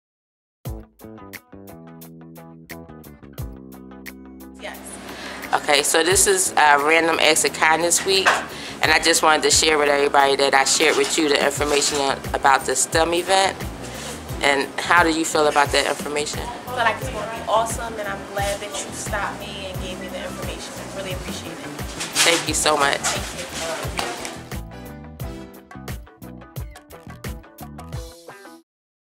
RAK2015STEM
i really didn't know what i was going to do for random acts of kindness week, but small acts became available and i did several random acts of kindness this week!! i video taped a brief interview yesterday with art for justice co founder Ann Marie Kirk and today i over heard a fourth grade teacher Sonia Blount talking @ dunkin doughnuts and i kindly interrupted their conversation to inform Ms. Blount about Pyramid Stem 2015 and ClassCraft!!